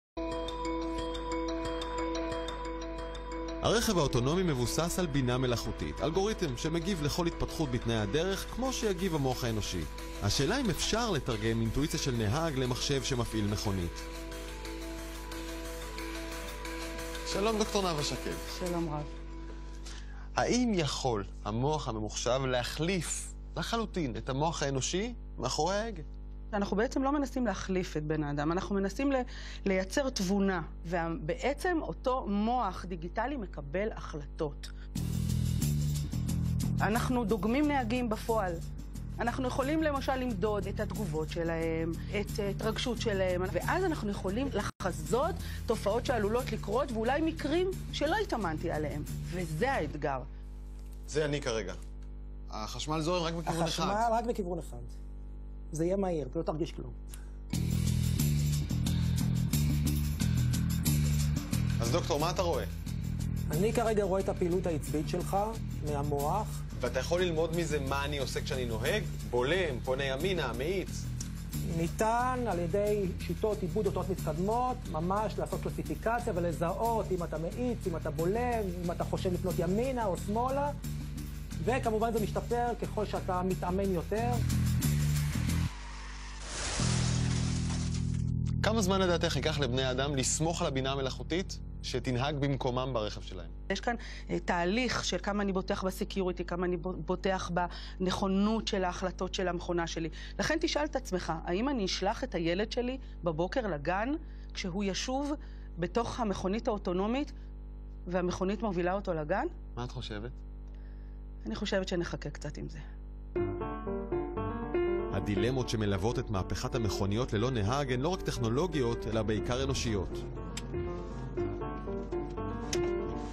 ד"ר שקד וד"ר שושניק בערוץ 2
סרט ששודר בערוץ 2 על מכוניות אוטונומיות בהשתתפות ד"ר נאווה שקד מומחית לבינה מלאכותית וד"ר רונן שושניק חוקר מוח.